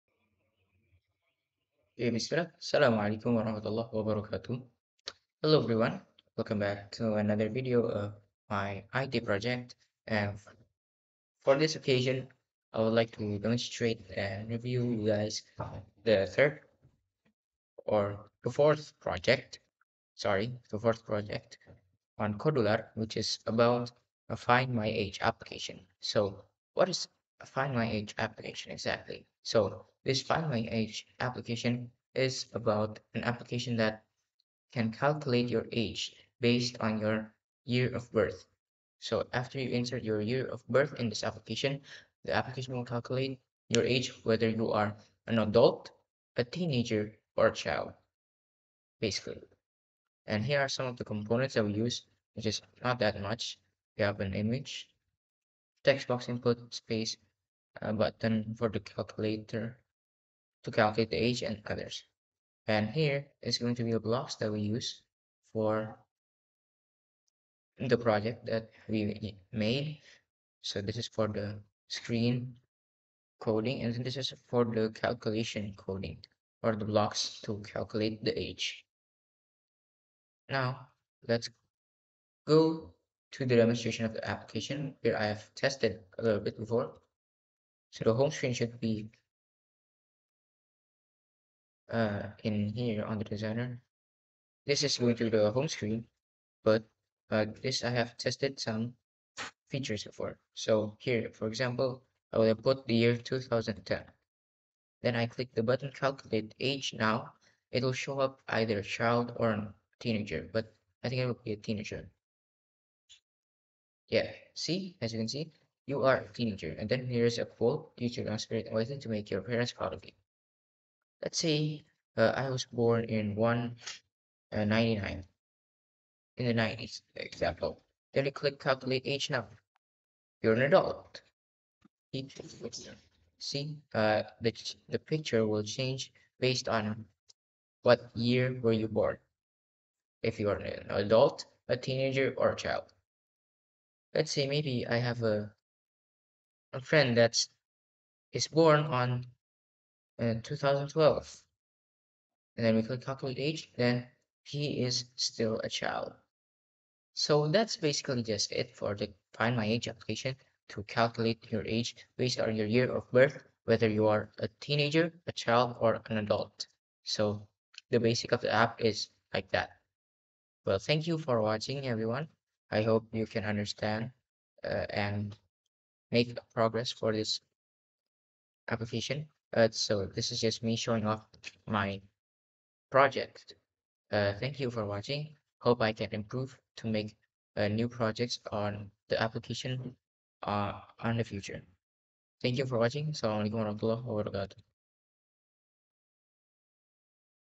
Find My Age App Demo | Kodular
Hello everyone, my name is Faishal Ahmad Rabbani, and you can call me Faishal. This video is my new video.
This video will review and demonstrate my fourth project in 9th grade, which is Find My Age App.